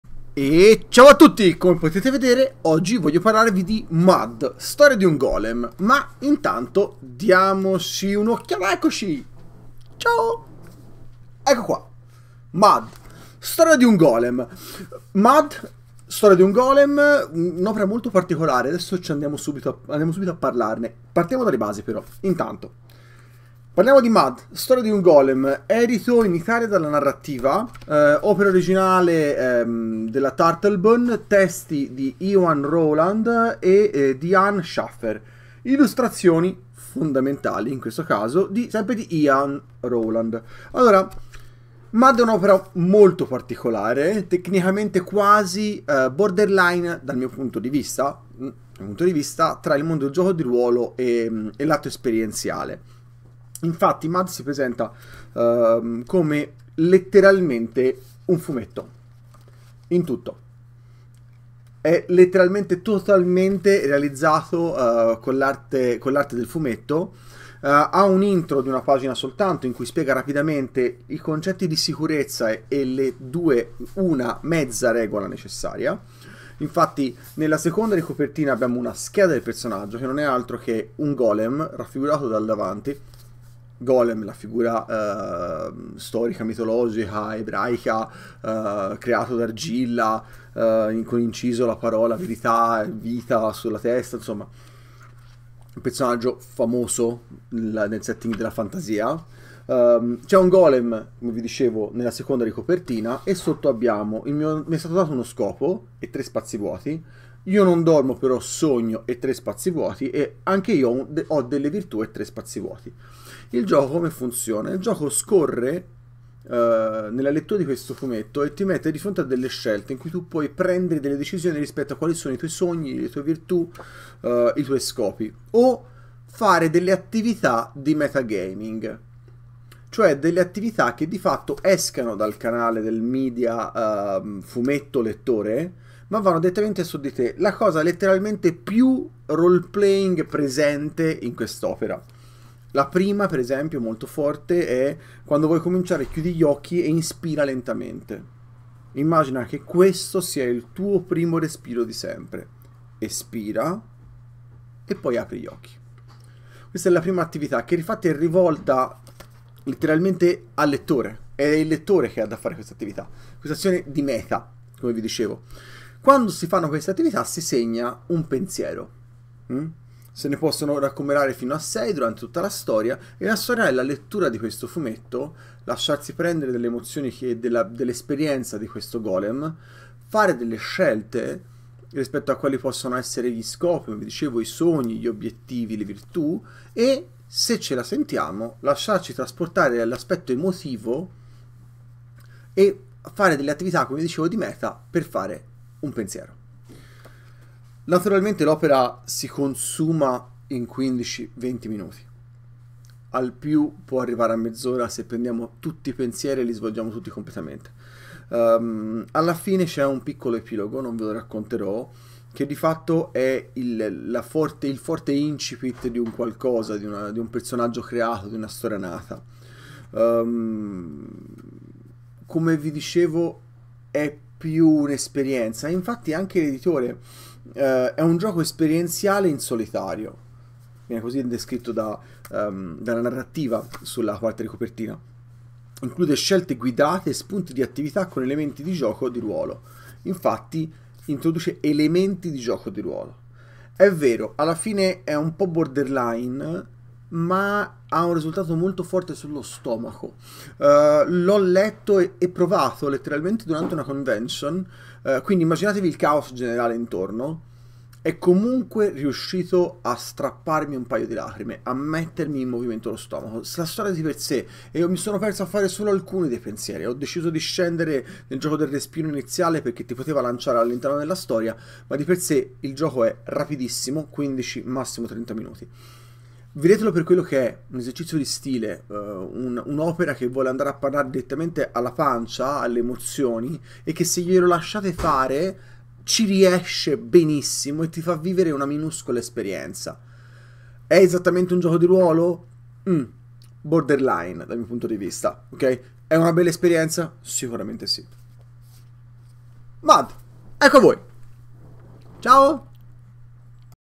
MUD storia di un Golem di Rowland & Shaffer (Ed. Narrattiva)
Un Fumetto con toni da GDR che smuove le melme nella pancia e le emozioni nel cuore :)